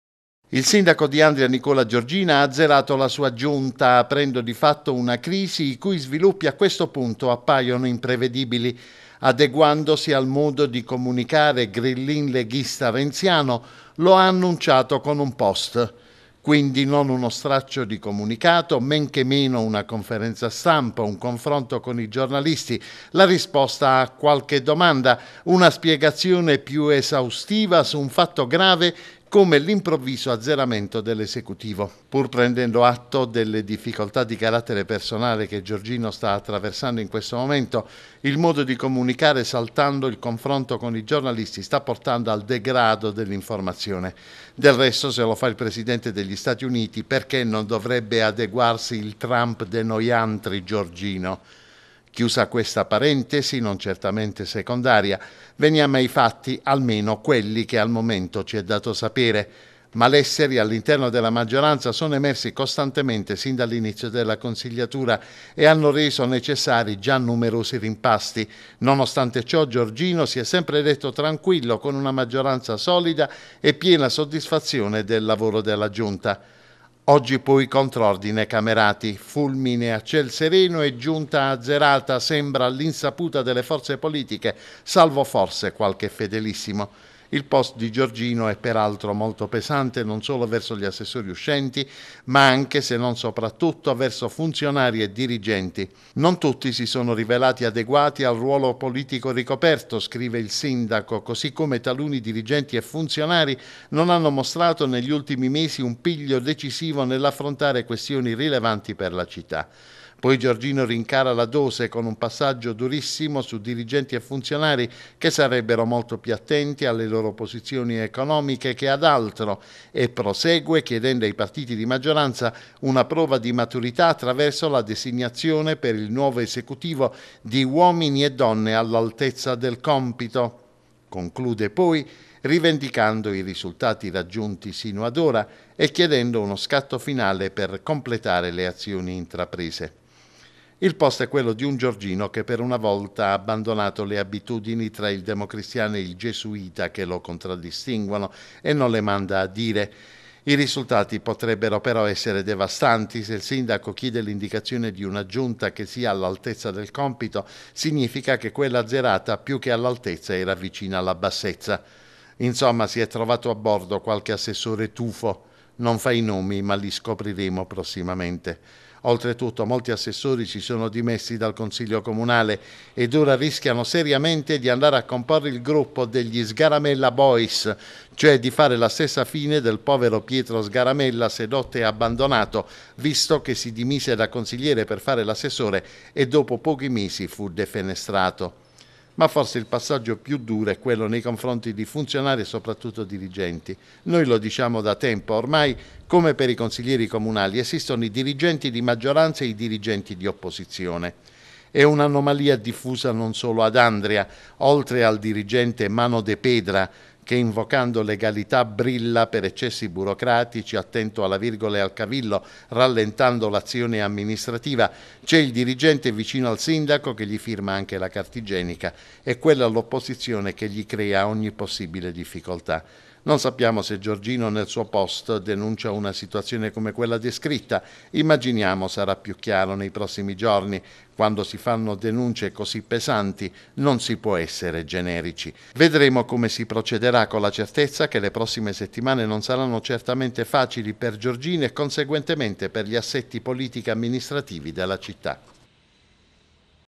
ANDRIA | Il sindaco azzera la Giunta e bacchetta i dirigenti: il commento di Roberto Straniero
Azzerata la giunta di Andria. Ad annunciarlo, questa mattina, è stato il sindaco Nicola Giorgino attraverso un post sulla sua pagina Facebook. Il servizio di Roberto Straniero.